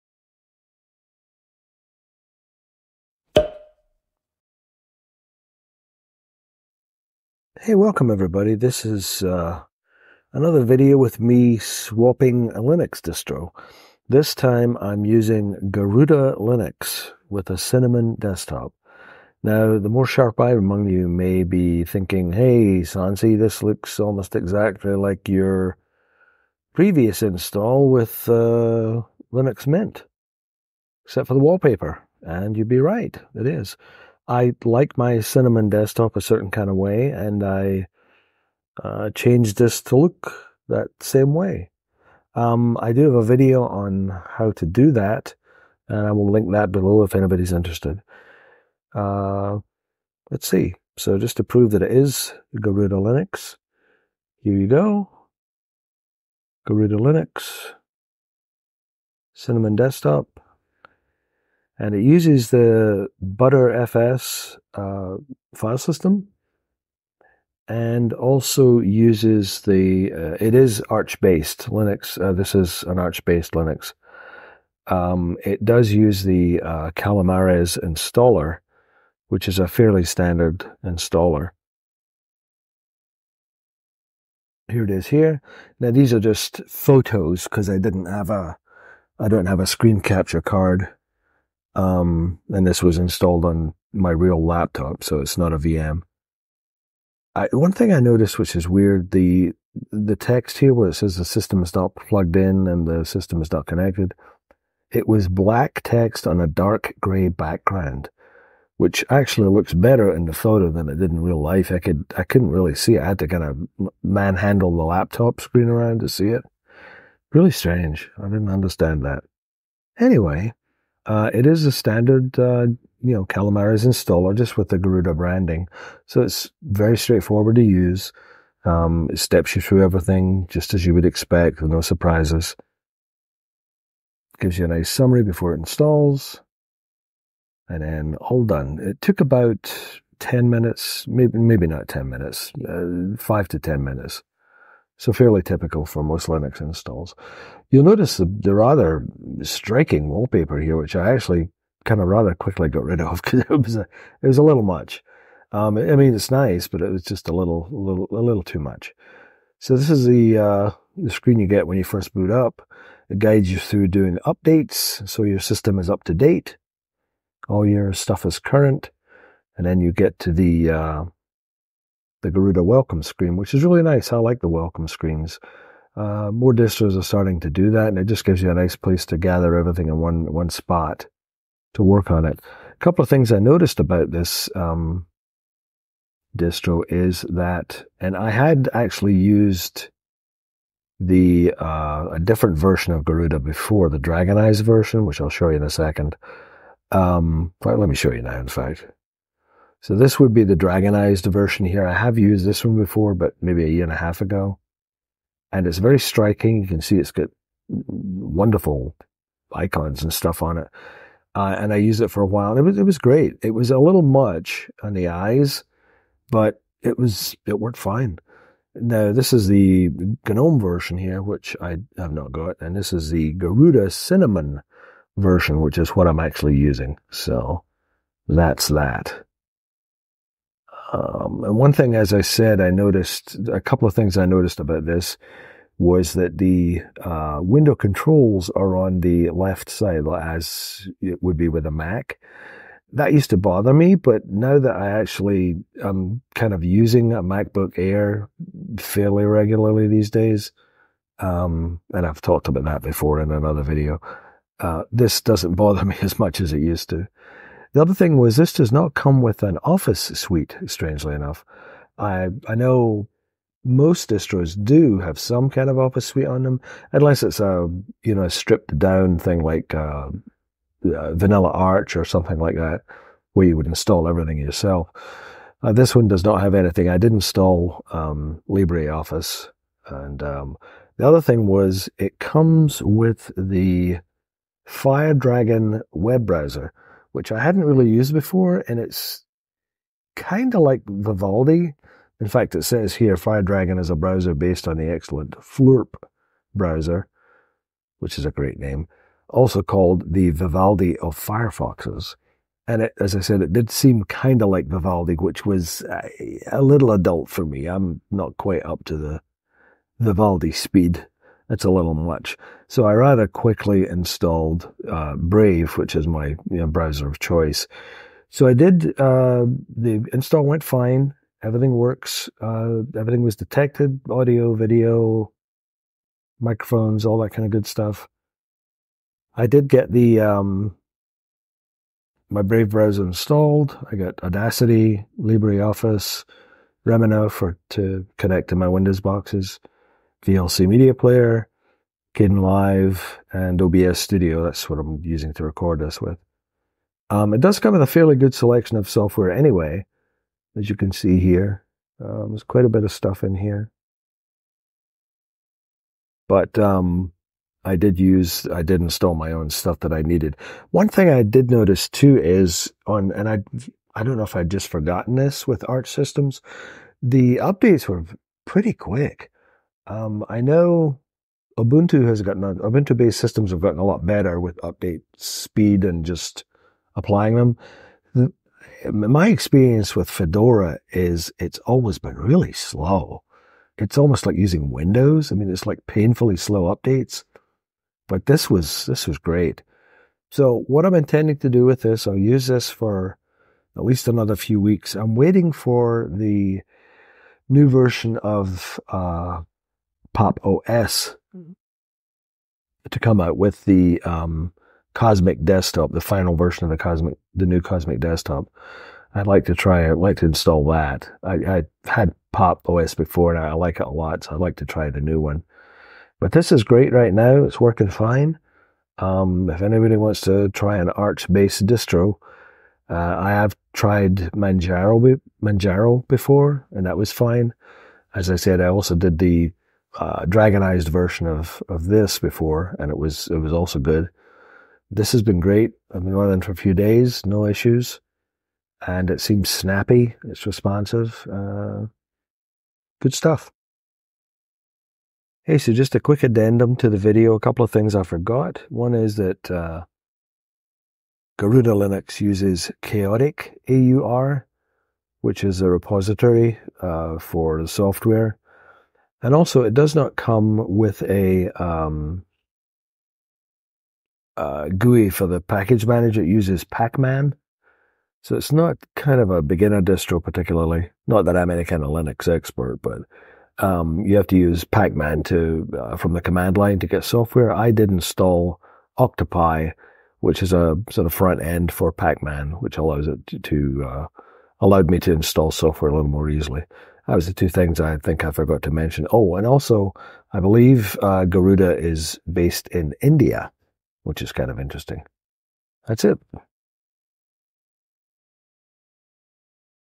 Looking at Garuda Linux Cinnamon
Another distro swap for yours truly. This time around it's Garuda Linux, which is an Arch-based distro.